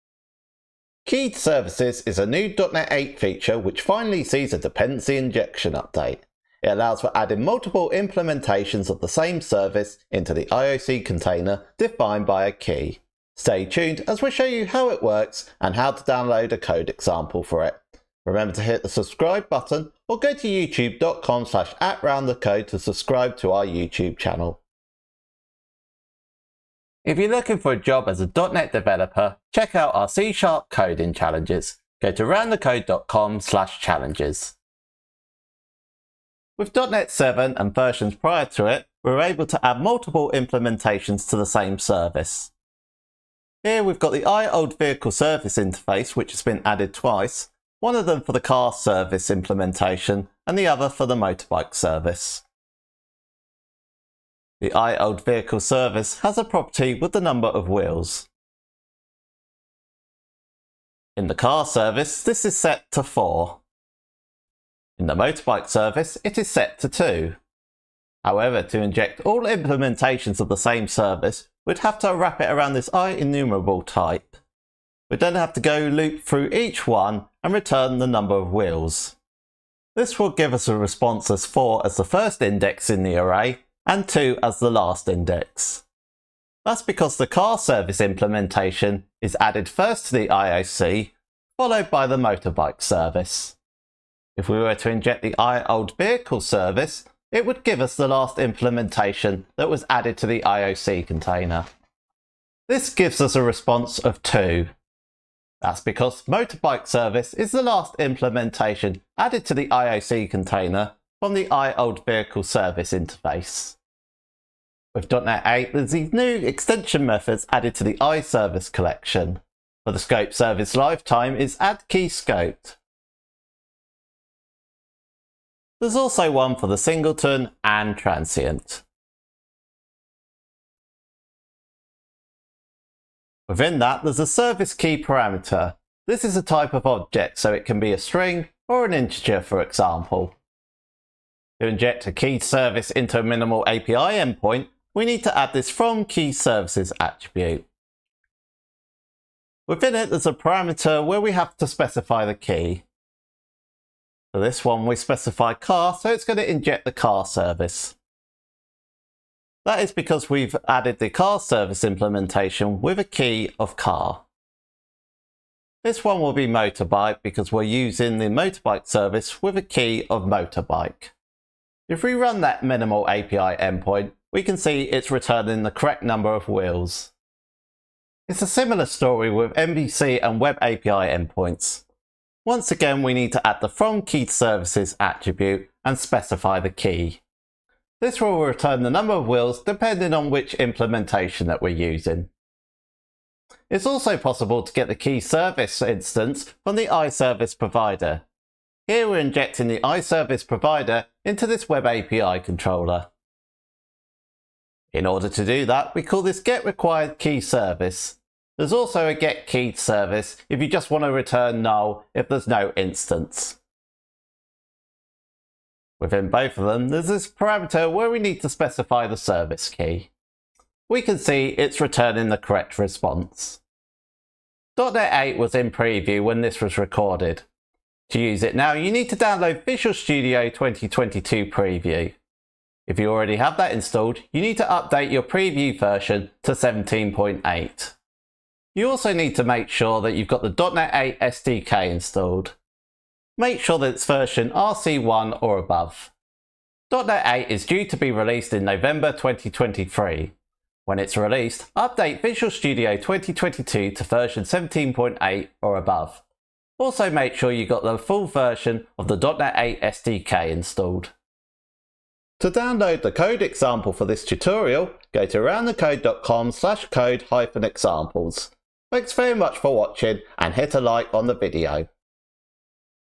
Keyed services sees an invaluable update for dependency injection
Keyed services is a new .NET 8 feature that sees an ASP.NET Core update to dependency injection.

It allows for adding multiple implementations of the same service into the IoC container defined by a key.

It works with MVC, Minimal API and Web API endpoints and can be configured with singleton, scoped and transient service lifetimes. As well as that, a keyed service can also be injected from the IServiceProvider instance thanks to a new method.

We'll show you how keyed services work and how you can start using them now.

► Chapters
0:00 What is a keyed service?
0:26 .NET coding challenges
0:36 How to add multiple implementations to the same service in .NET 7
1:54 Keyed services extension methods in IServiceCollection
2:20 How keyed services work in Minimal APIs
2:57 How keyed services work in Web API and MVC
3:13 Inject a keyed service with IServiceProvider
3:44 How to use keyed services right now
4:21 Download the code example